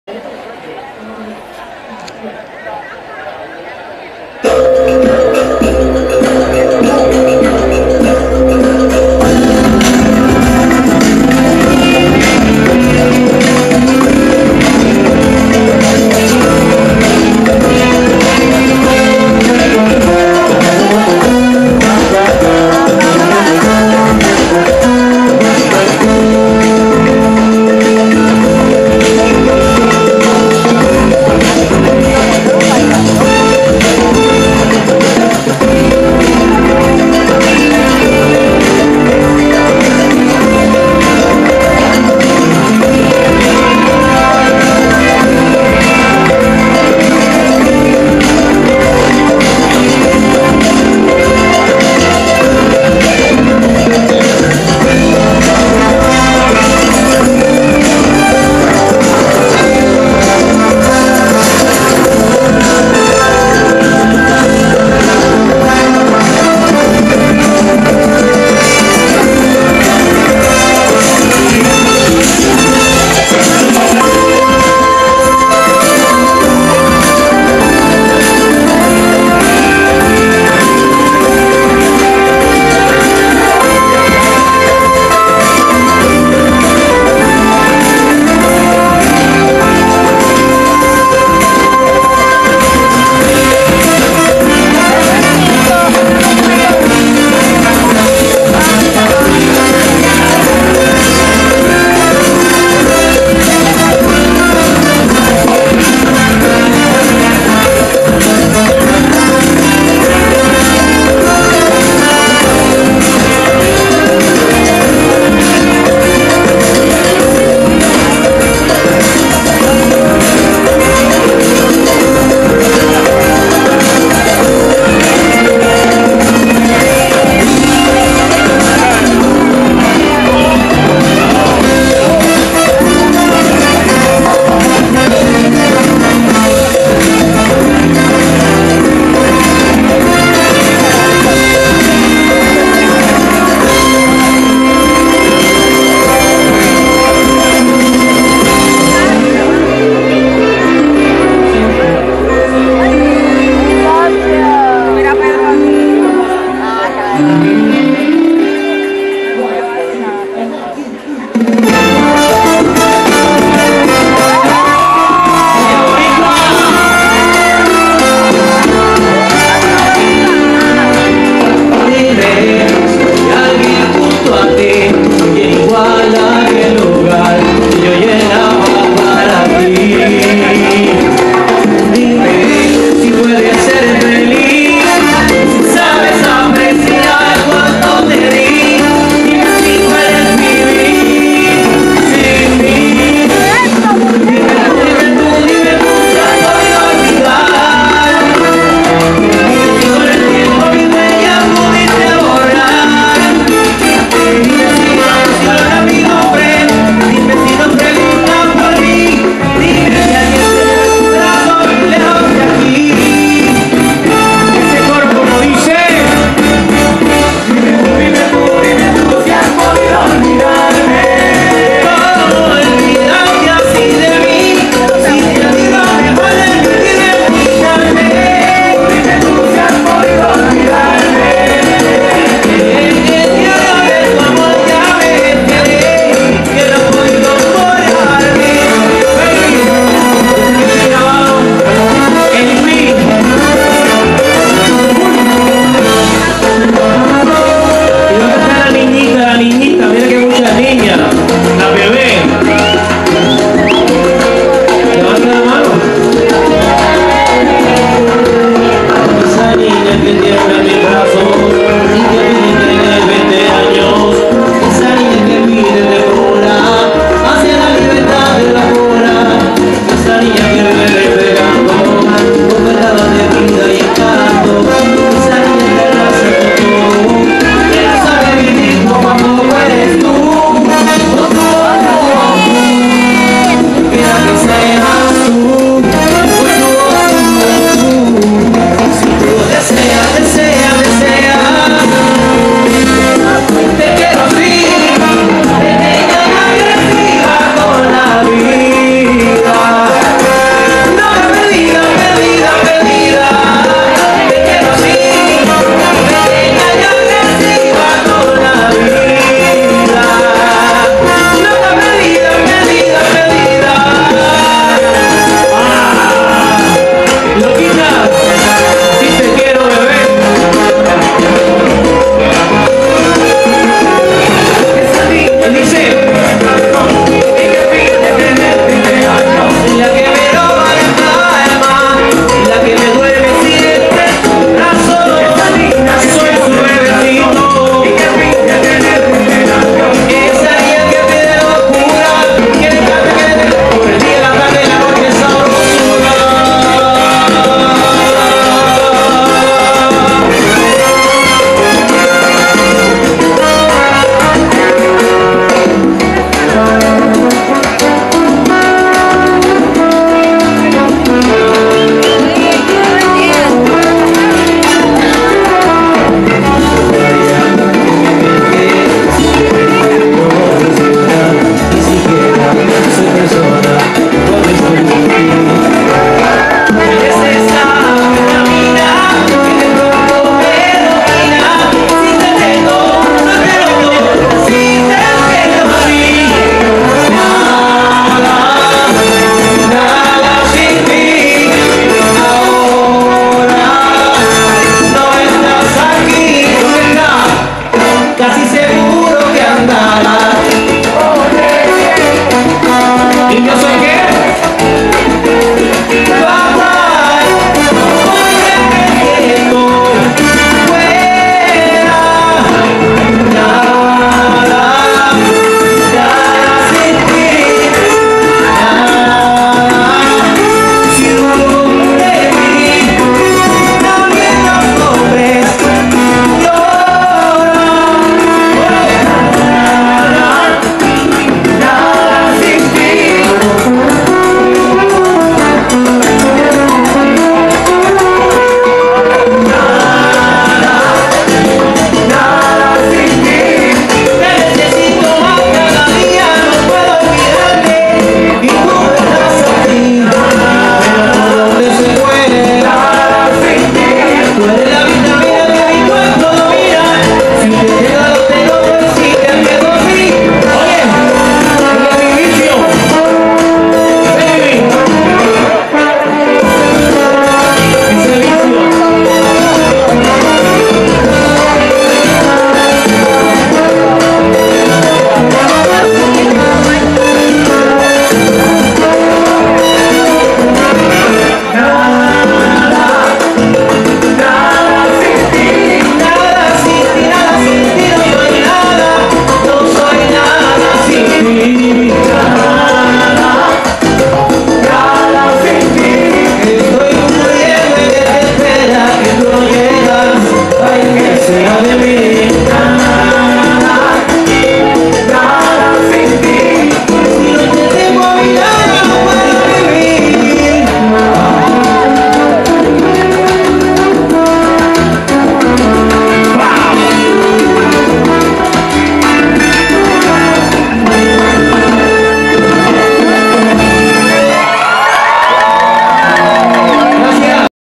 #1 JERRY RIVERA AT SALSA Y SAZON 2019
Latin festival at Dr Phillips Center in Downtown Orlando,fl(3/24/19)